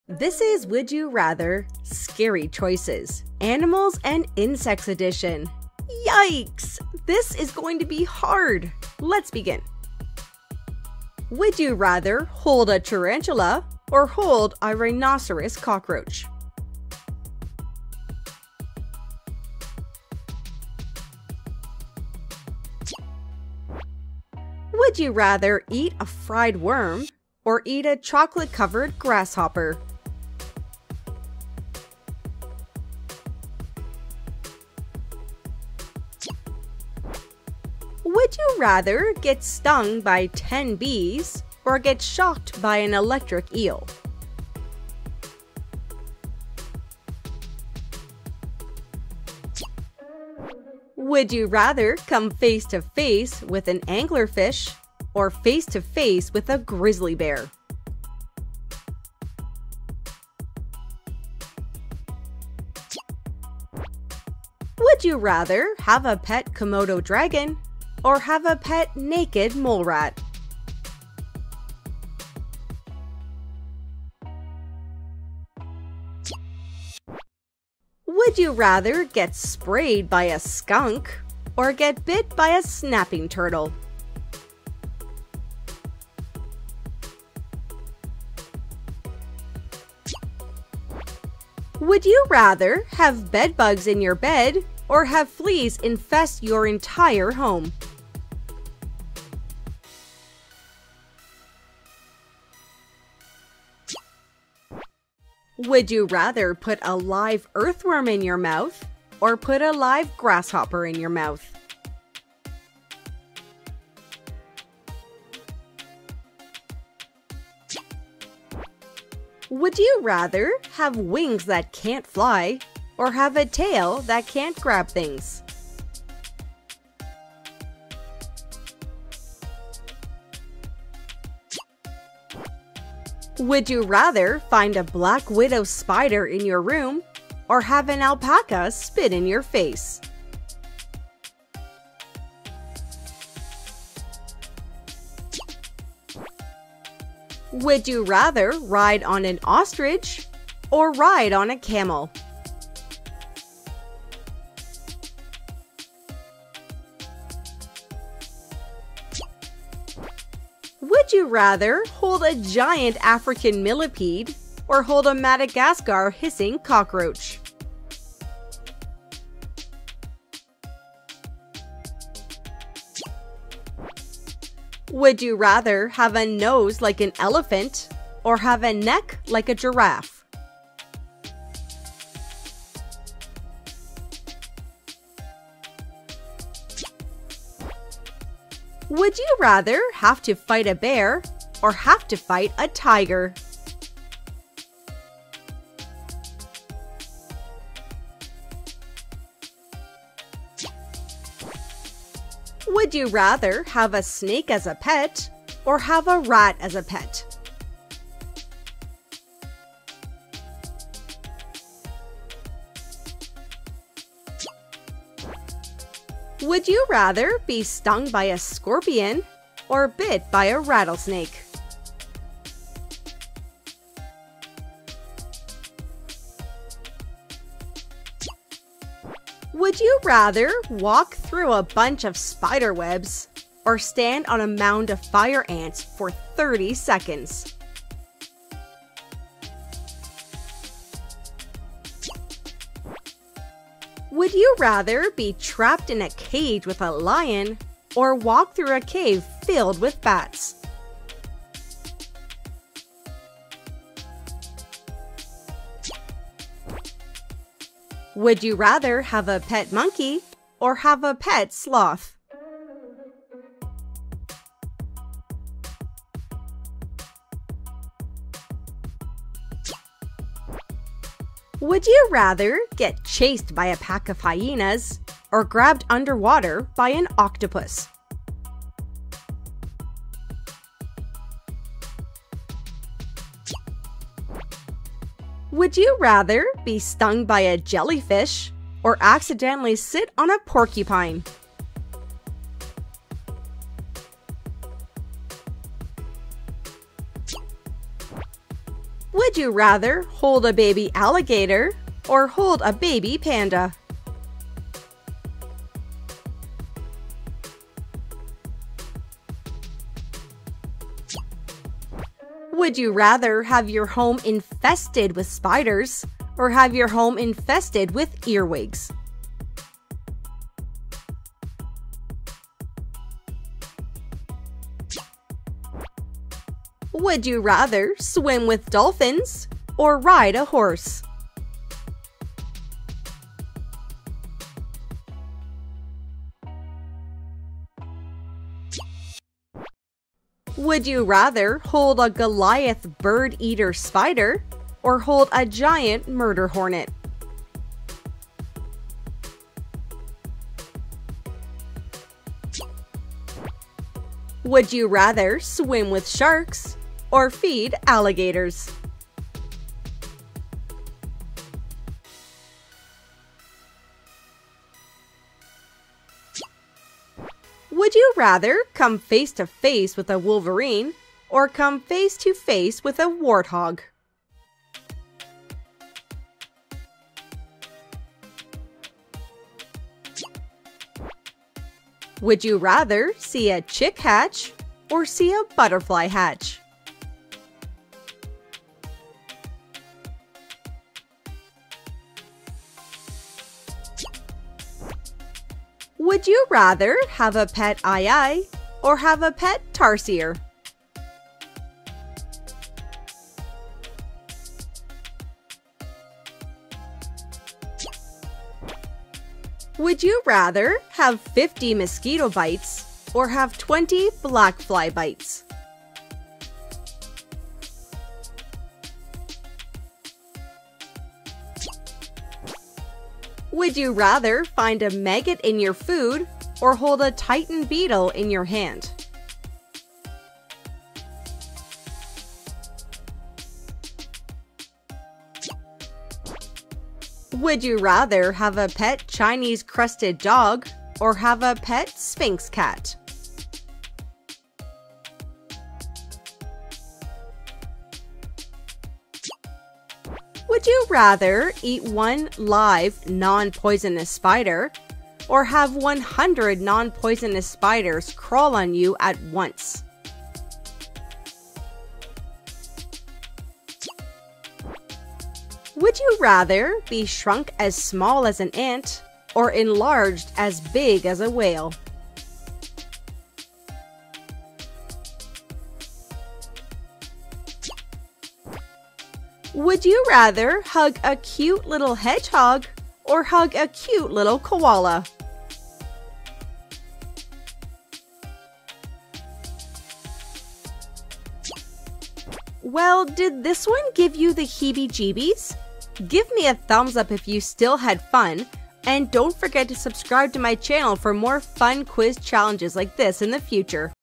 Would You Rather... Animals & Insects 🐛 🐶 (HARDEST Choices)
Get ready to choose between some of the hardest would you rather animal and insect questions you have ever had to before. I have prepared 40 of the hardest would you rather questions related to animals and insects, for you in this fun challenge.

In this would you rather animal and insect version that I have prepared, you are going to have to choose one of the options on the screen before the timer runs out. This won’t be easy, but remember, you must choose one of the options before the next question is asked.

I really hope you enjoy this version of Would You Rather, and if you did, a thumbs-up on the video would be greatly appreciated.